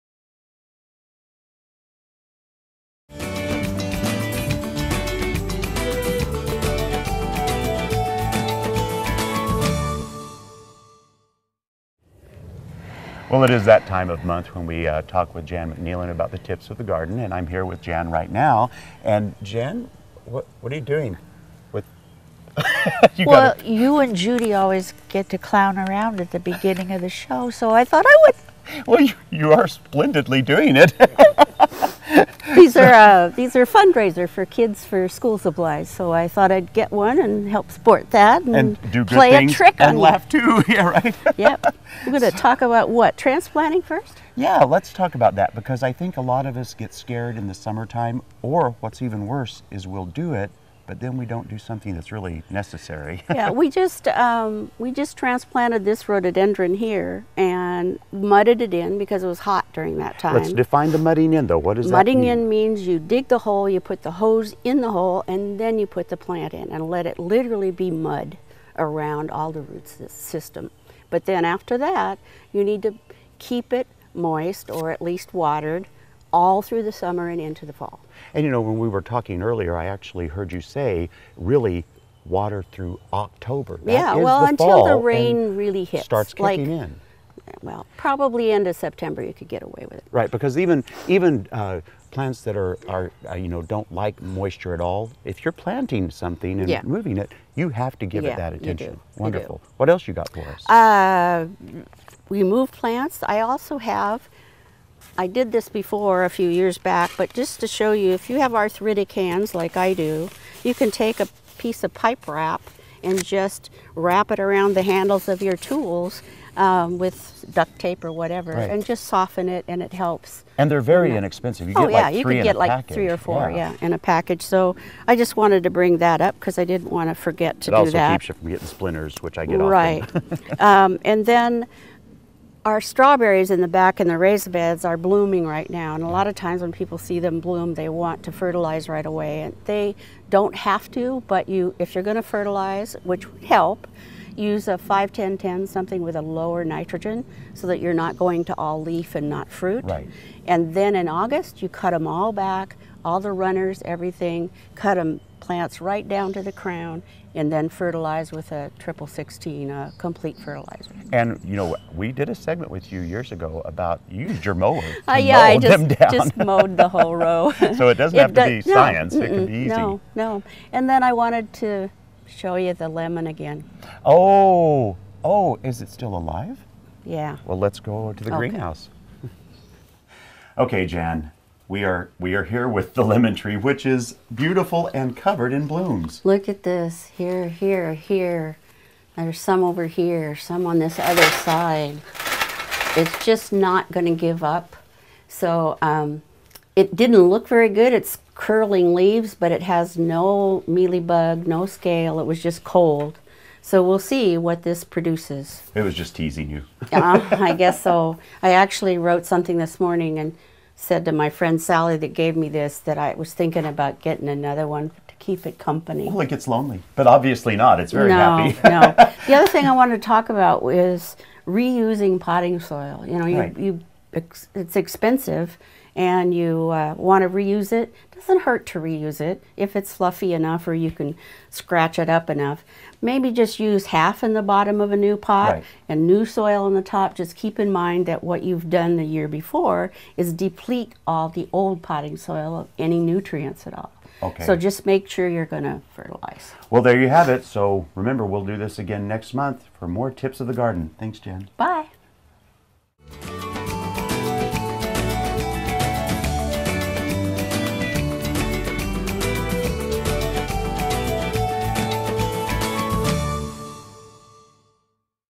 Jan's May Tips
It is time for the tips of the month. We talk about transplanting, tools for the elderly, a hardy lemon and tired soil.